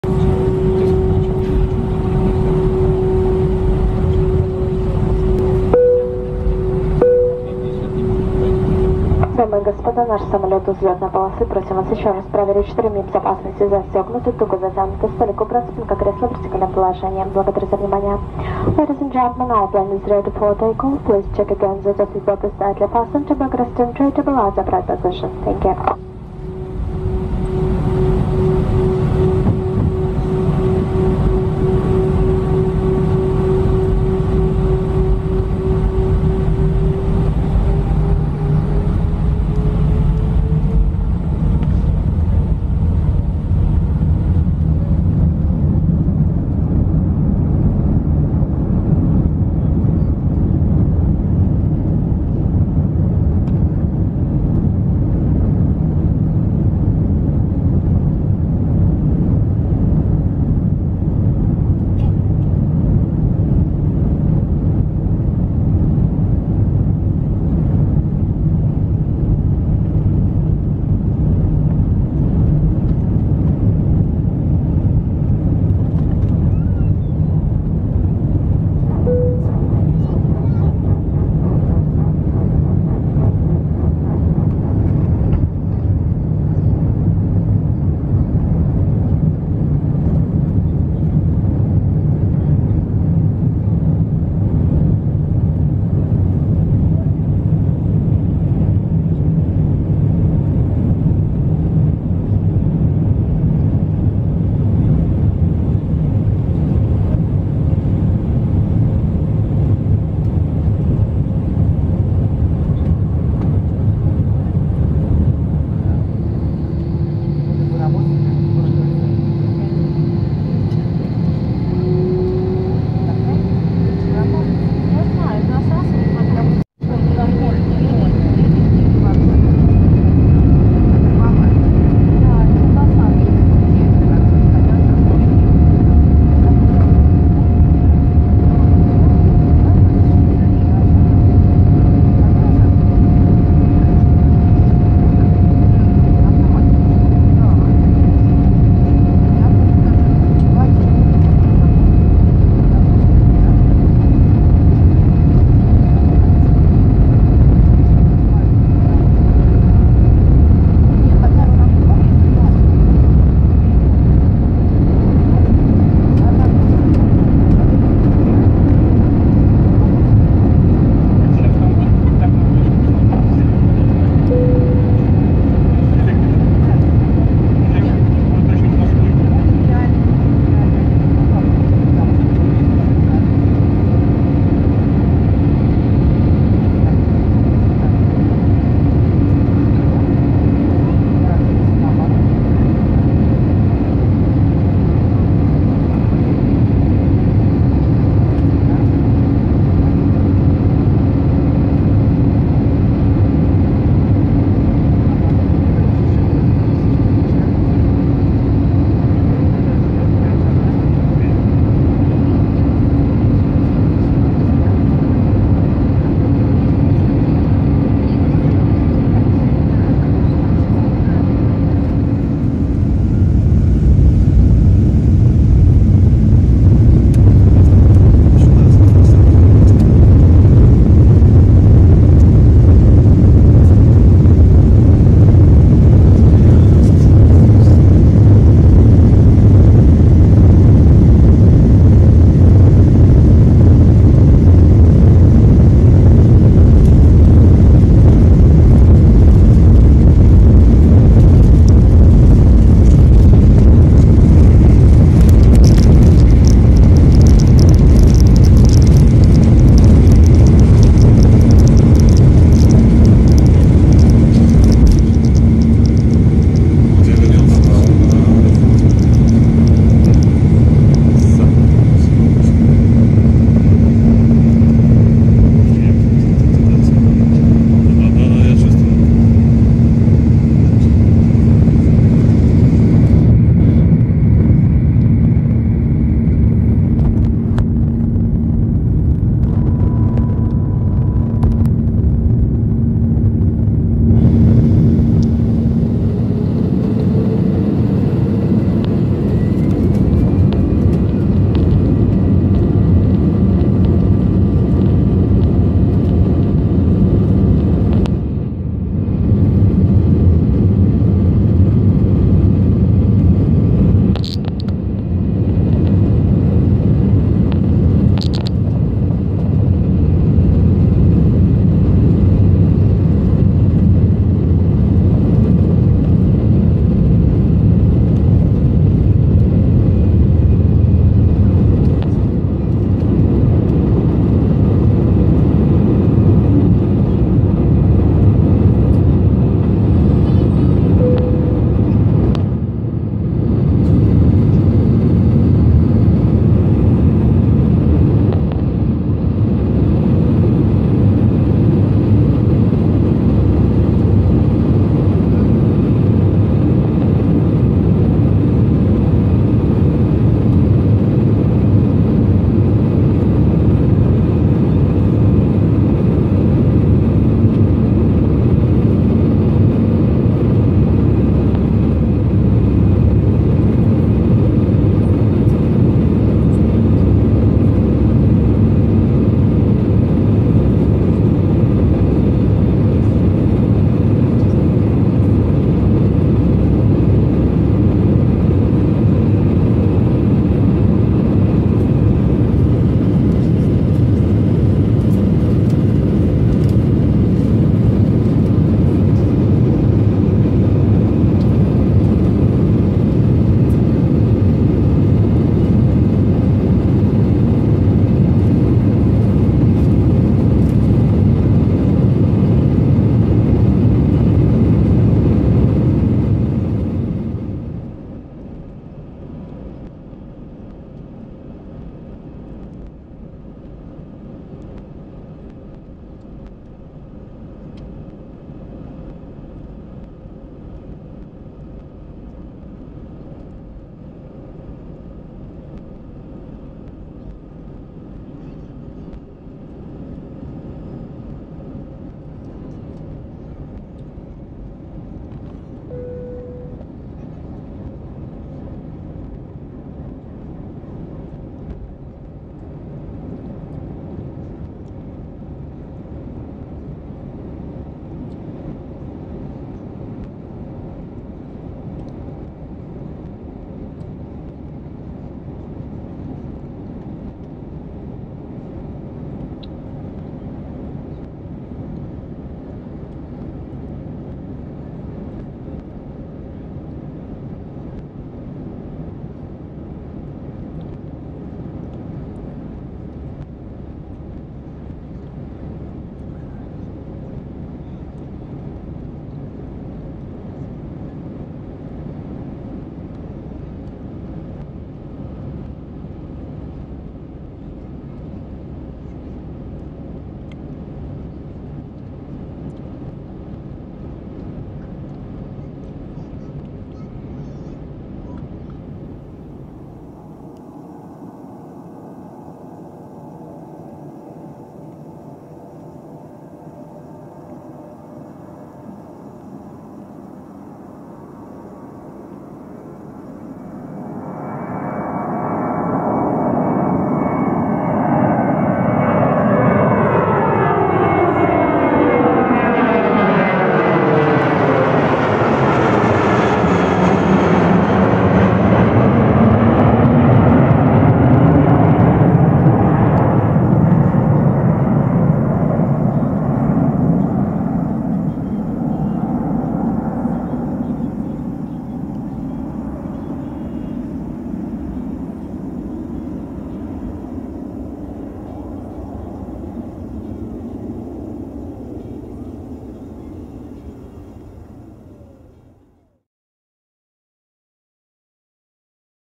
Takeoff Airbus A320.
Video about takeoffs of narrow-body aircraft Airbus A320. Take-offs take place from the airports of Sochi, Sheremetyevo, Ust-Kamenogorsk, etc.